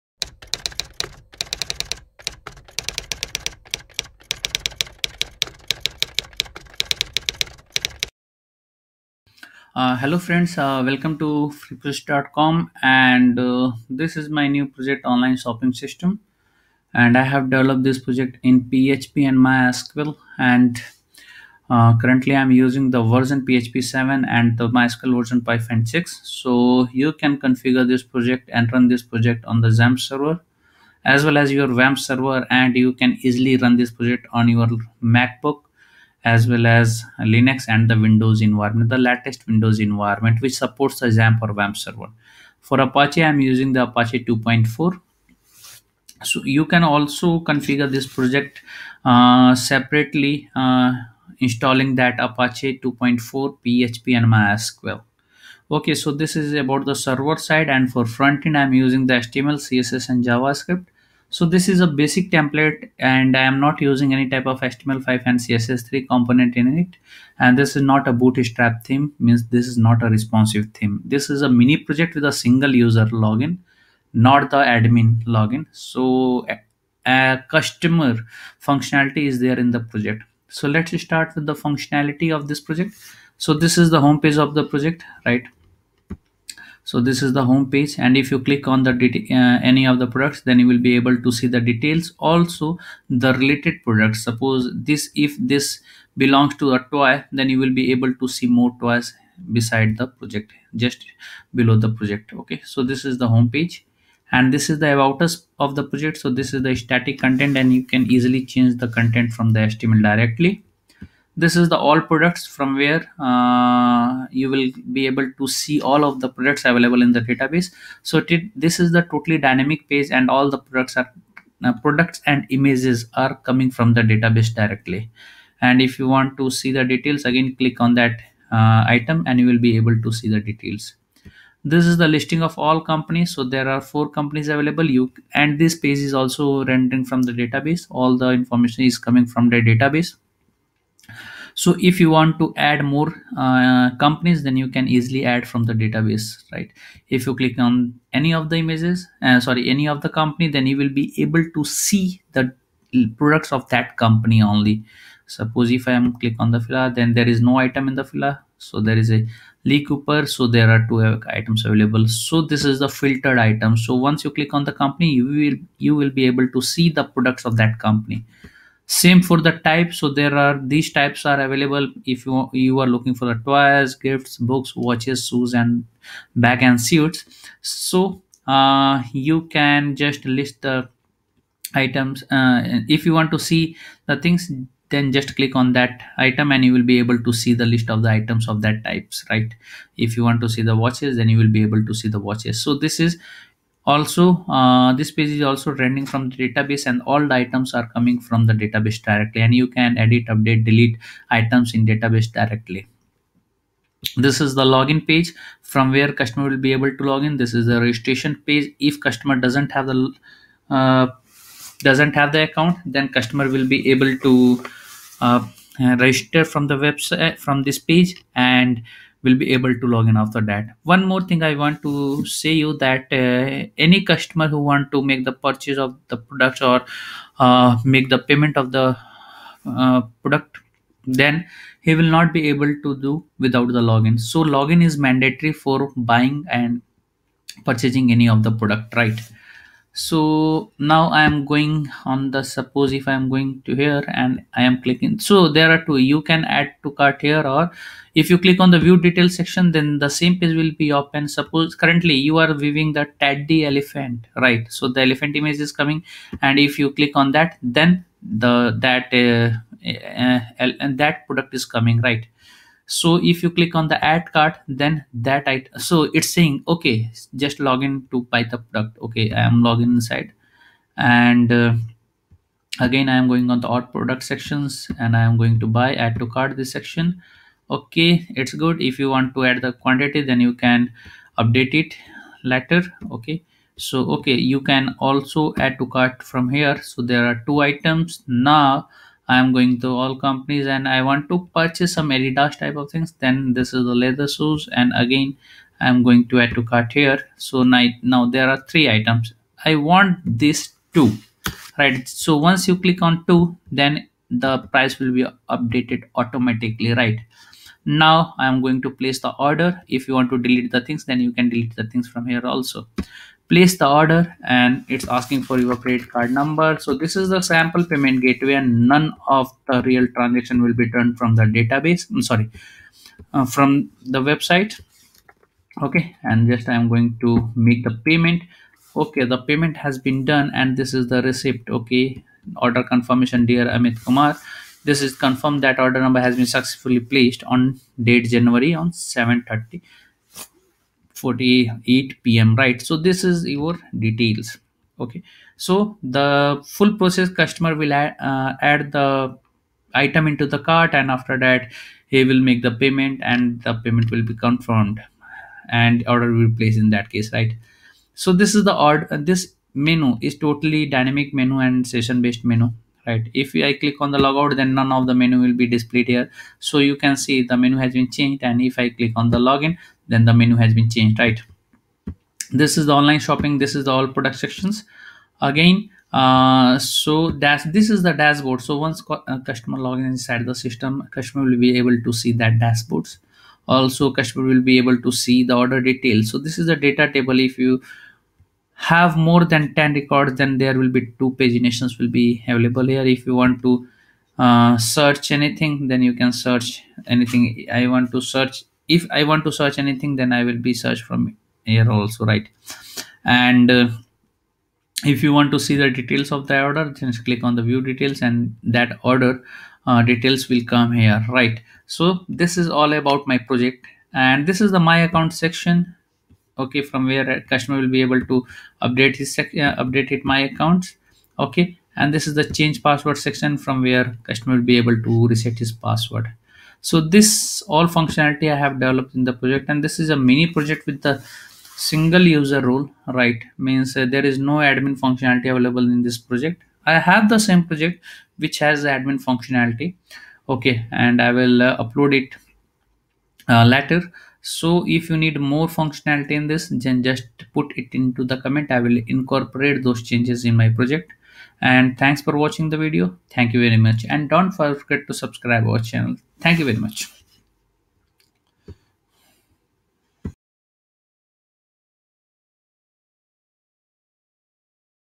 Online Shopping System | PHP and MySQL Project Source Code | PHP MySQL CRUD Project
Online Shopping System is a PHP MySQL micro service project where all APIs we have developed in PHP MySQL and all UI Online Shopping System  we have implemented in PHP.

Online Shopping System PHP and MySQL Project Tutorial
Online Shopping System Download